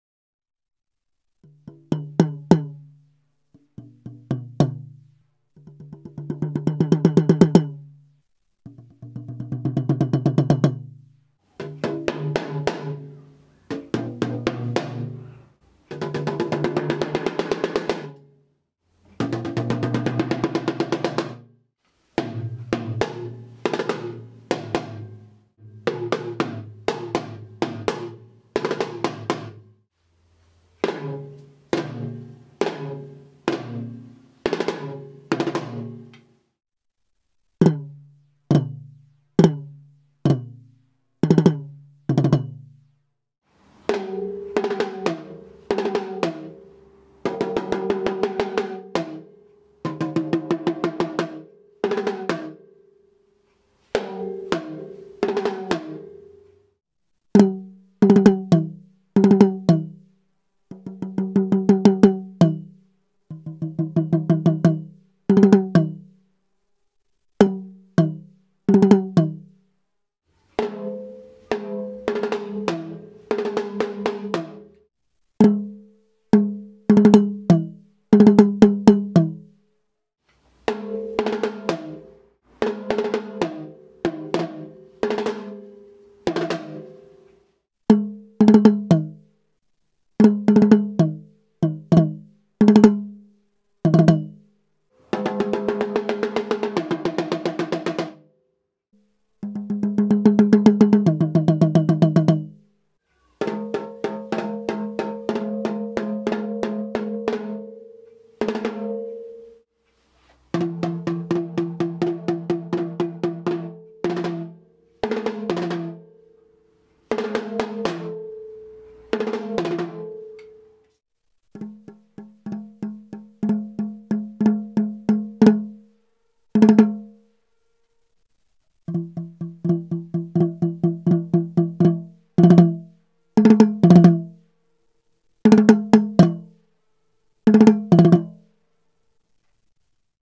Acrylic Octobans - Boss Custom Drums
These 6 inch diameter octobans are 9.5 and 13.5 inches deep. The standard thickness for acrylic drums of this diameter tends to be 3mm but I wanted to find out what thicker shells would sound like. These are 5mm thick, seamless acrylic shells.

They are fitted with Evans Level 360 clear G1 heads, triple flanged hoops and laser engraved acrylic badges.

The video switches between the camera mic and the close mics (MB75s placed just inside the shell). There no no audio effects added at all.